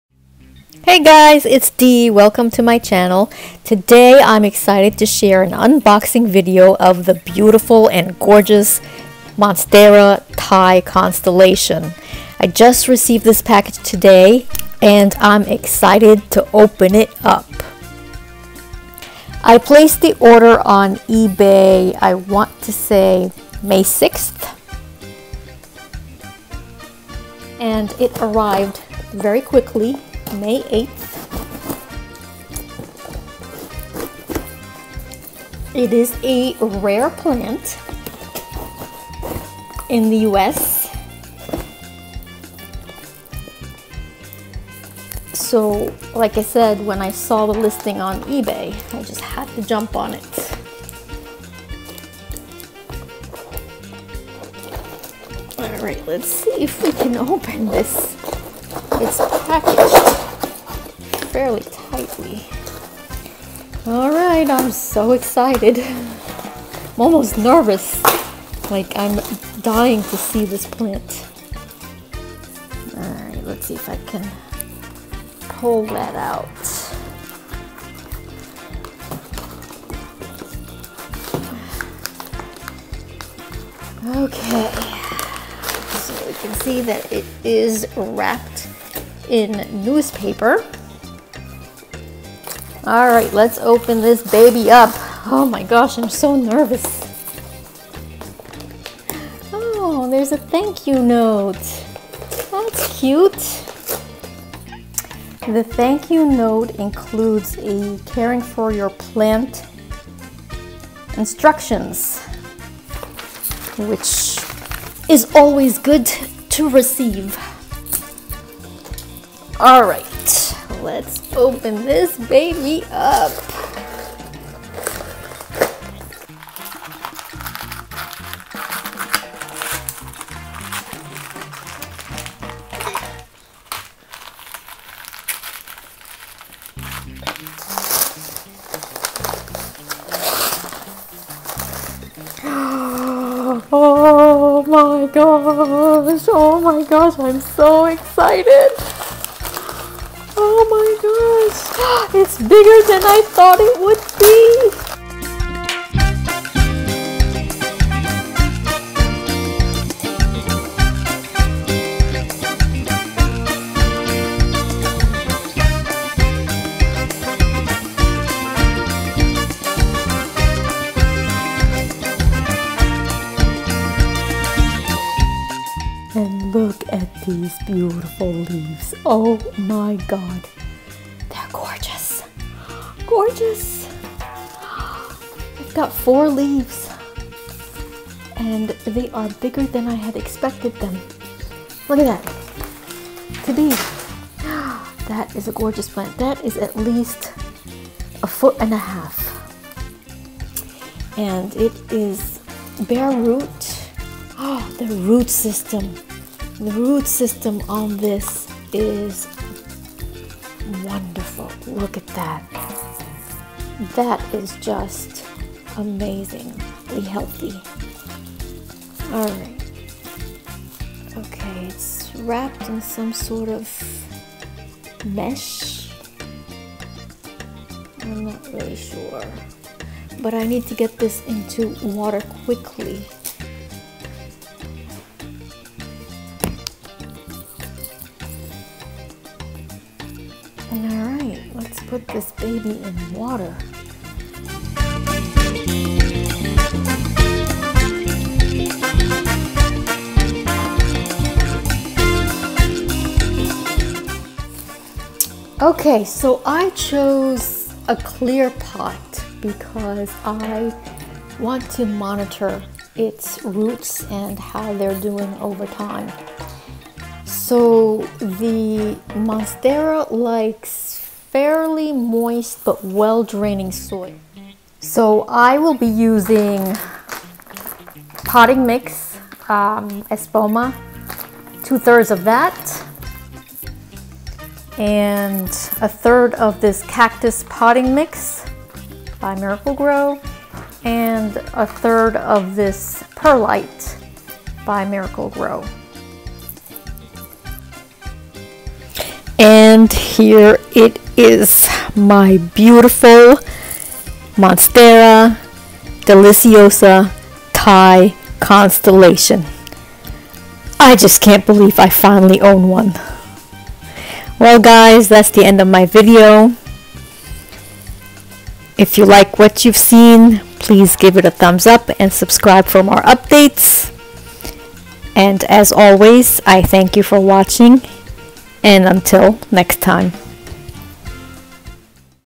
Rare Houseplant Unboxing - Variegated Monstera Deliciosa "Thai Constellation": May 8 2019
Join me as I unbox the latest plant I've added to my houseplant collection, the rare (and very hard to get in the US) Variegated Monstera Deliciosa "Thai Constellation”.